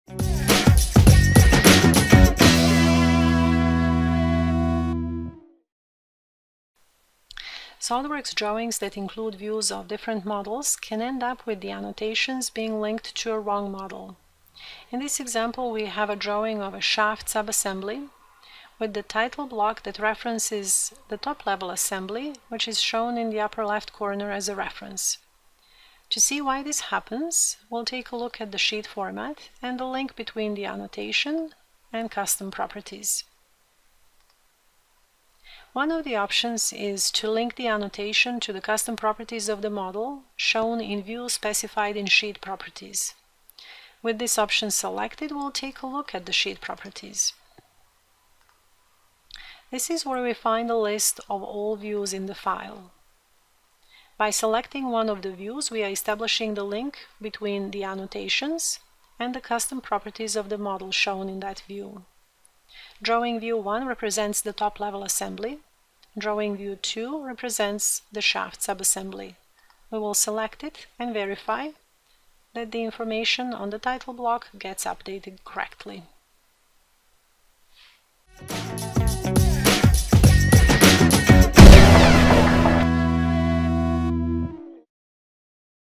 SOLIDWORKS Tutorial - Troubleshooting and Linking Annotations to Models
In a SOLIDWORKS drawing that references multiple models, annotations sometimes get linked to a wrong part or assembly.

In the example shown here, the title block refers to the top-level assembly that is displayed in the upper left corner as a reference, and not the shaft assembly that is shown with a BOM and balloons.

This is how to fix it.